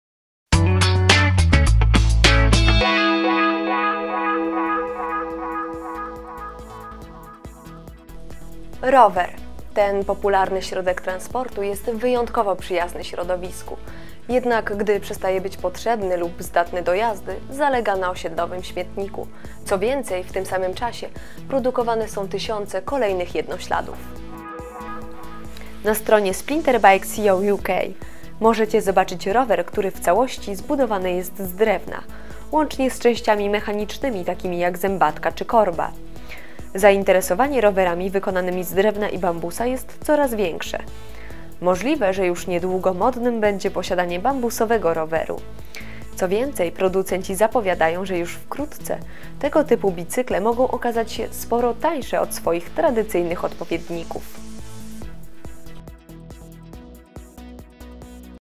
Drewniany rower
Rower - popularny środek transportu wyjątkowo przyjazny środowisku. Zobacz rower wykonany w całości z drewna.